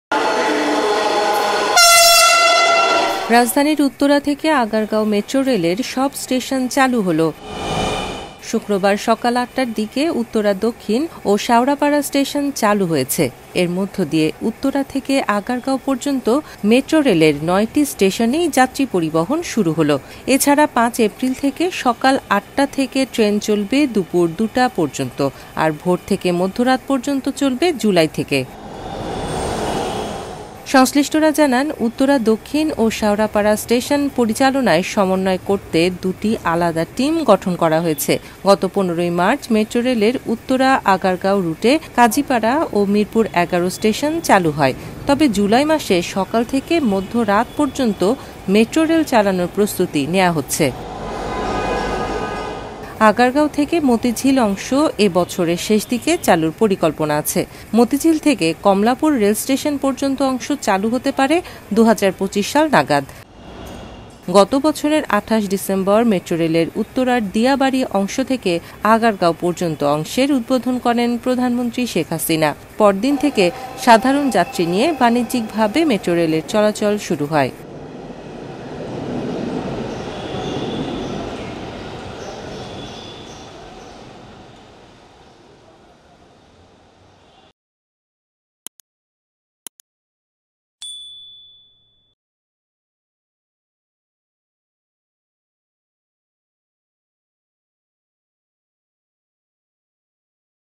মেট্রোরেলে যুক্ত হলো আরও দুটি স্টেশন | Dhaka Metro | Protidiner Bangladesh
মেট্রোরেলে যুক্ত হলো আরও দুটি স্টেশন | Dhaka Metro | @ProtidinerBangladesh

»» Office Address:
Rongdhanu Corporate,